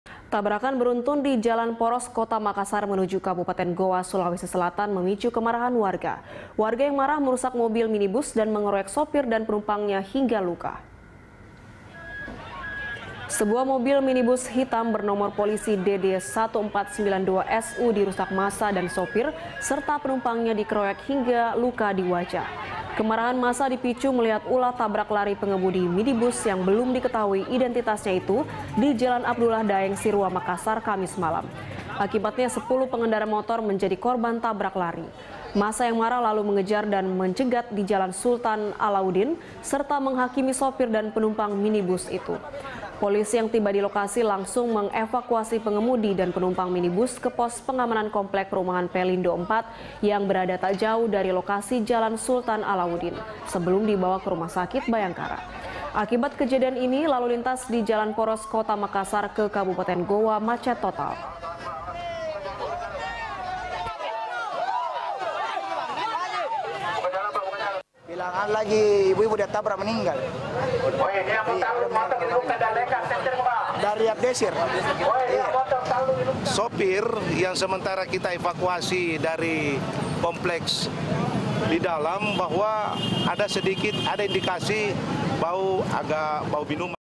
Tabrak Lari, Dua Pria di Makassar Diamuk Massa
Tabrakan beruntun di Jalan Poros Kota Makassar menuju Kabupaten Gowa, Sulawesi Selatan memicu kemarahan warga. Warga yang marah merusak mobil minibus dan mengeroyok sopir serta penumpangnya hingga luka.

Facebook.com/BeritaSatu
Youtube.com/BeritaSatu